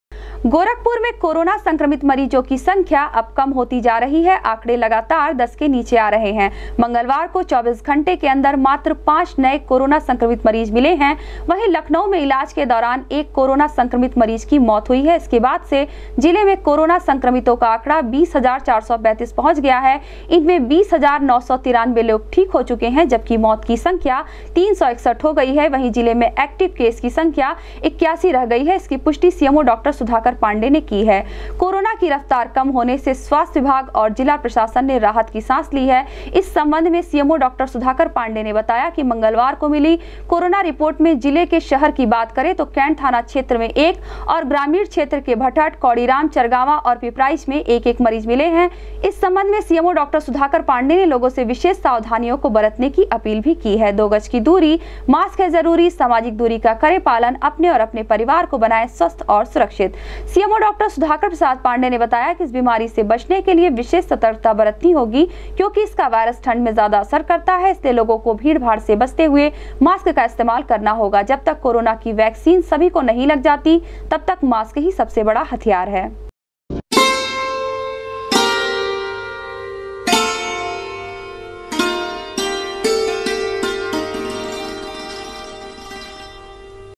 गोरखपुर में मंगलवार को मिले मात्र 5 नए कोरोना संक्रमित मरीज, एक की हुई मौत, जिले में कोरोना संक्रमित
गोरखपुर में मंगलवार को मिले मात्र 5 नए कोरोना संक्रमित मरीज, एक की हुई मौत, जिले में कोरोना संक्रमित मरीजों की संख्या हुई 21435, अब तक 361 कोरोना संक्रमित मरीज गवां चुके हैं अपनी जान, 81 रह गए एक्टिव मरीज, सीएमओ डॉ सुधाकर पाण्डेय ने की पुष्टि